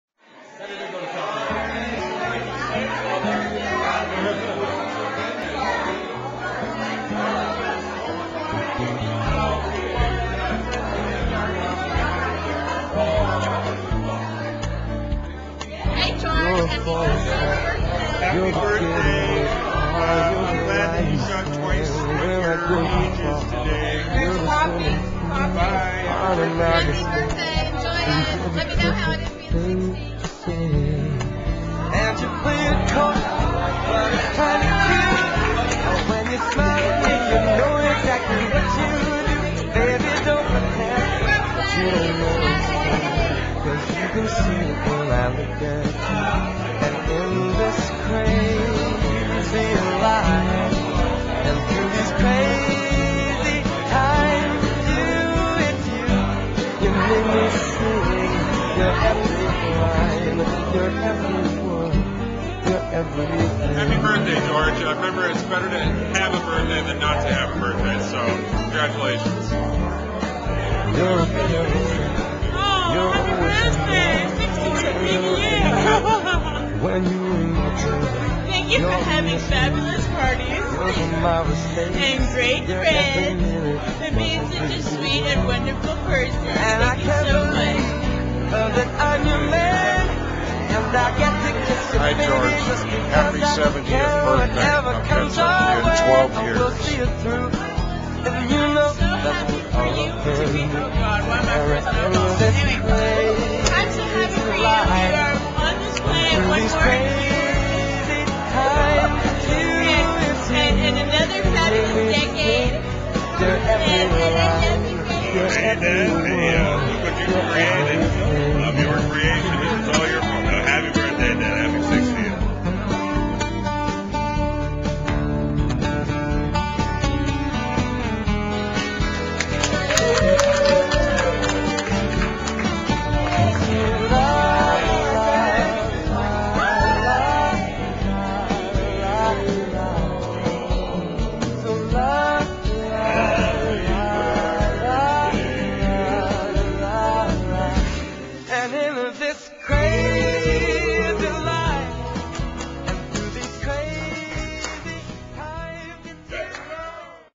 George Reynolds 60th Birthday!
George Reynold's 60th birthday bash! Held at George and Dona's   Midtown home in Houston..good pizza, good wine, great friends, priceless memories!